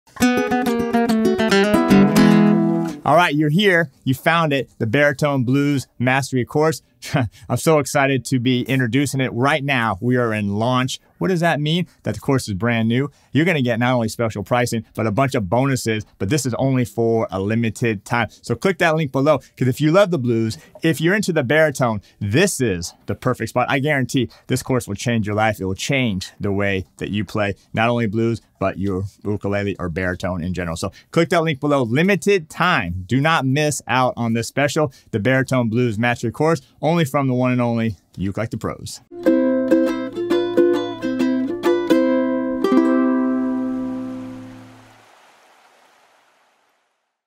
Brand New ULTP COURSE!!! Baritone Ukulele Blues Mastery Course!!!
Hey guys!!! I'm so happy to finally announce that we have our BRAND NEW Baritone Ukulele Blues Mastery Course!

The Baritone Blues Mastery course is going to transform the way you play and understand music. This one course will not only improve your technique, knowledge, and enjoyment of the Baritone ukulele but you will see and hear markable growth in your playing. The knowledge you will learn will not only help you become Baritone Blues Master but It will also make you better at your strumming, rhythm, technique, feel, and confidence.

You will get 15 Baritone Blues Lessons with Step by Step Instruction and also the chance to download Fast and Slow Backing Tracks and printable PDF TAB/Sheet Music, 6 week of the Baritone Q&A, the Free PDF download of the baritone blues mastery book and so much more bonuses!!!

JUST FOR A FEW DAYS GET THIS INCREDIBLE NEW COURSE WITH THE SPECIAL LAUNCHING PRICE!!

4) READY TO BUY A UKULELE?

6) ARE YOU INTERESTED IN GUITAR?

Terry